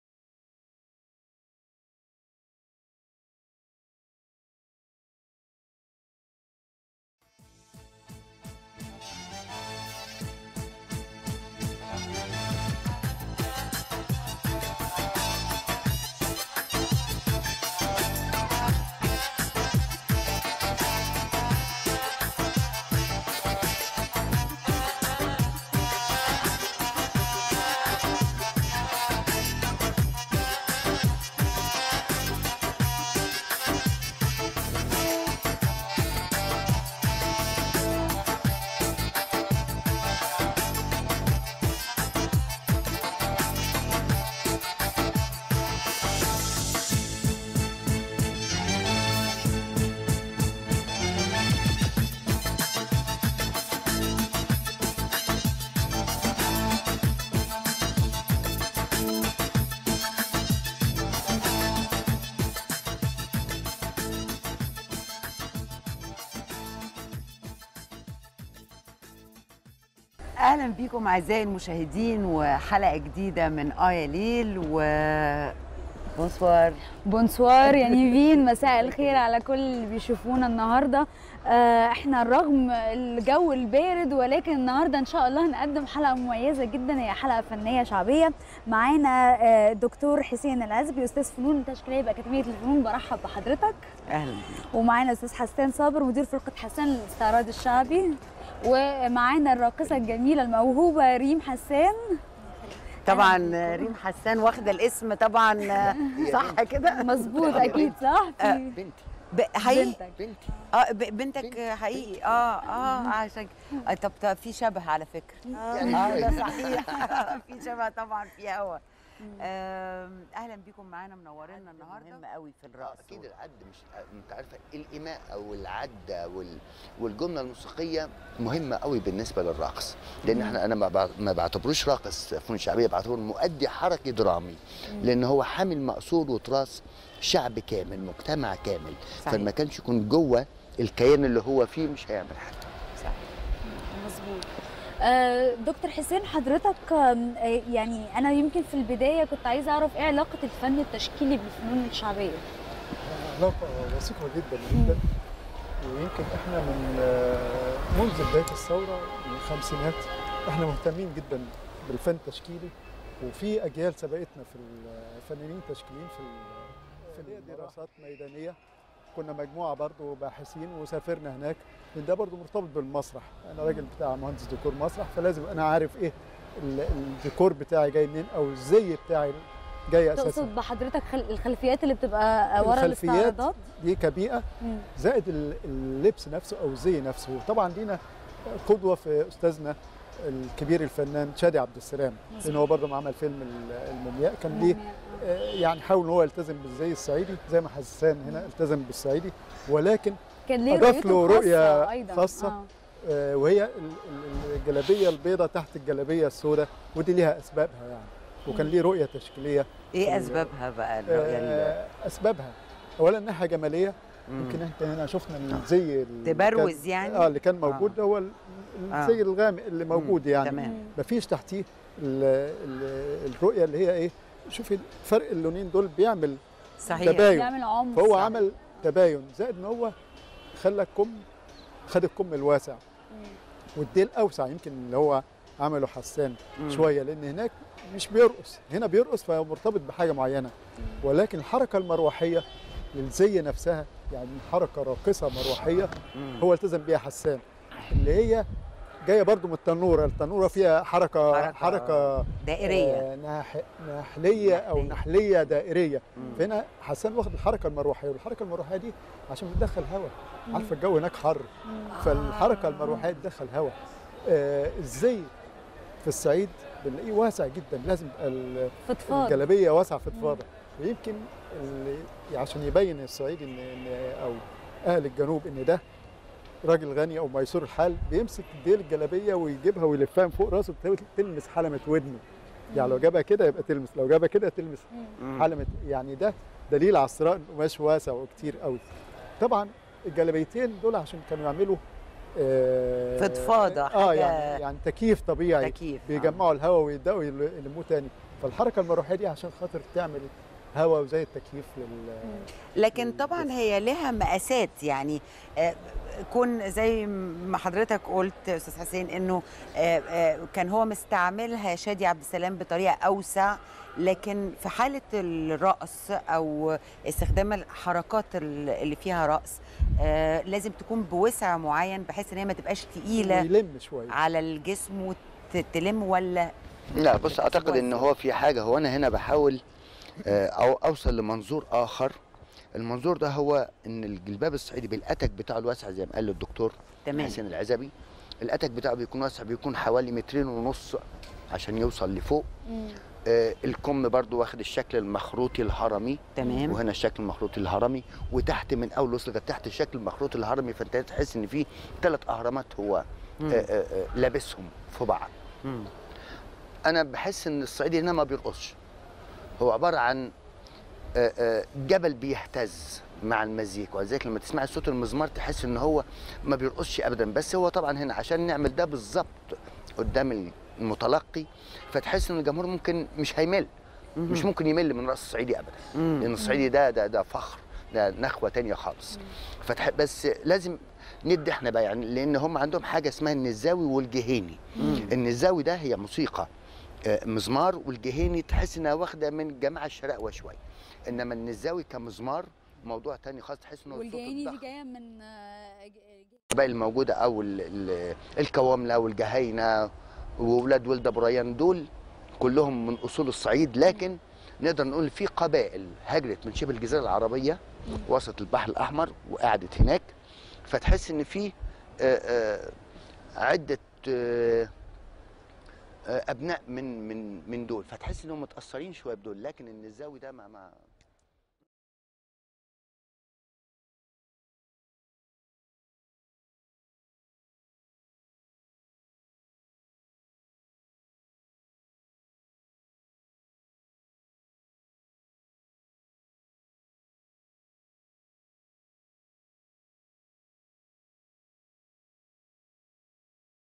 Hassaaniyat Folk حسانيات الفلكلورية About dancing in Upper Egypt
TV interview
On the Egyptian official television
On the Nile TV channel
With Hassaan Saber and Dr. Hussein El-Ezaby
About dancing in Upper Egypt